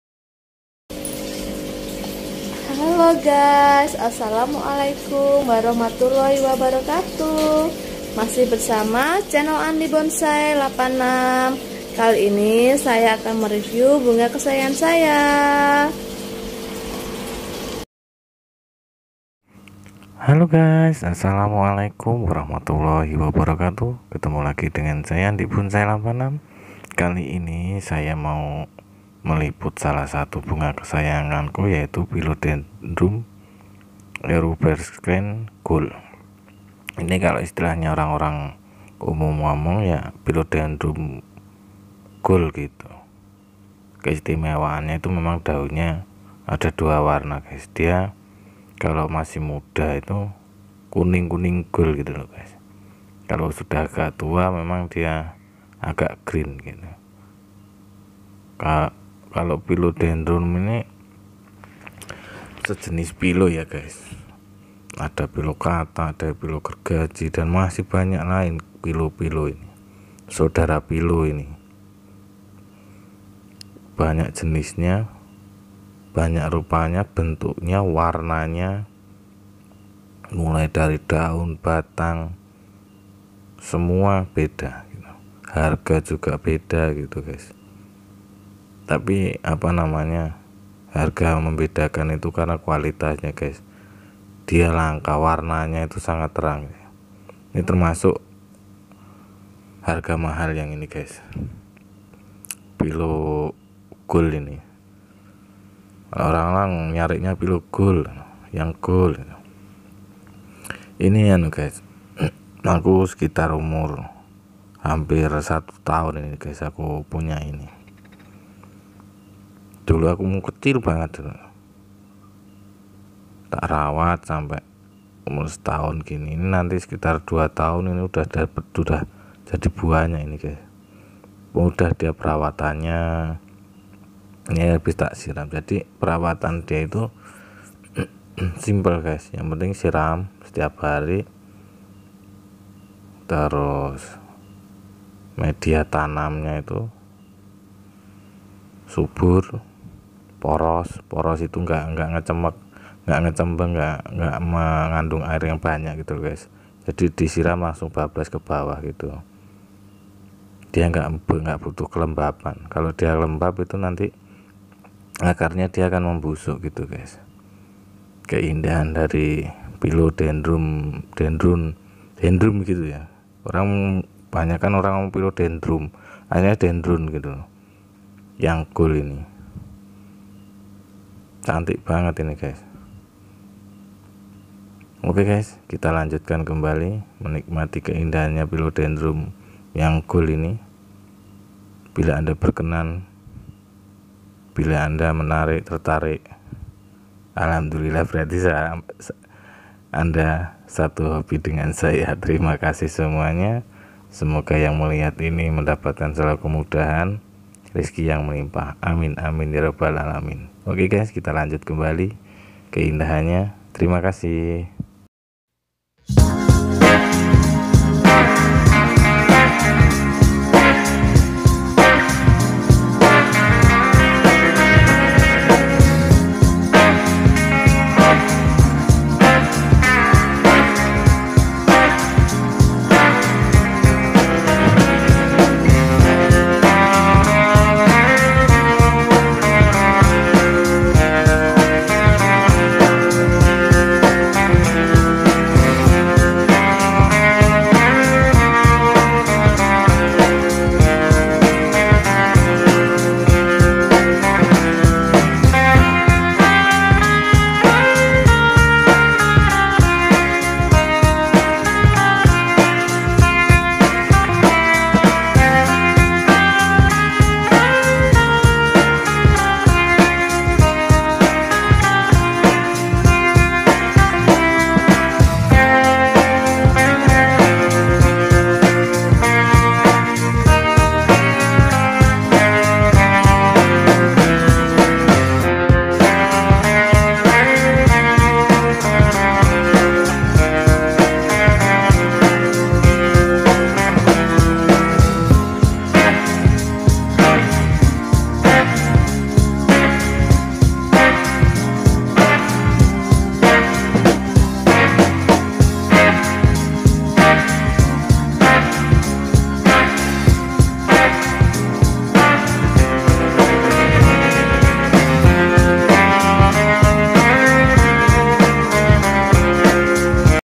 philodendron erubescens gold pert1
philodendron erubescens gold
Philodendron erubescens, philodendron yang memerah, adalah spesies tanaman berbunga di keluarga Araceae, asli Kolombia. Ini adalah pemanjat pohon cemara yang kuat yang tumbuh hingga 3-6 m, dengan batang merah dan daun berbentuk hati dengan panjang hingga 40 cm.